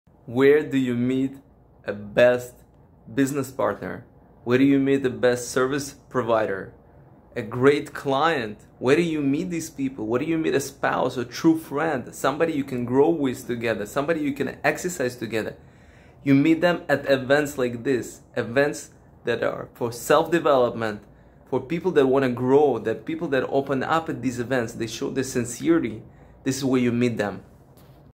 where to meet new quality friends, find a spouse, or a business partner???
Where do you find like-minded, growth-oriented friends, partners, clients, and service providers?! At self-development events like this one!!!
It's time to take action! Learn tools & strategies that will enable you to drastically improve your health, finance, productivity, and spirituality. Based on the ancient wisdom of Torah & Jewish sages and modern-day experts like Jim Rohn, Stephen Covey, Tony Robbins, Brian Tracy, Napoleon Hill, Jim Collins and many others.
During this one-day personal transformation seminar we will plan our year, dance, network, think, feel and learn how to bring out best self!

grow with purpose: $20 of general admission and $50 of VIP tickets will go to support our brothers and sisters impacted by war in Ukraine. We will also be conducting a short public prayer for the peace in Ukraine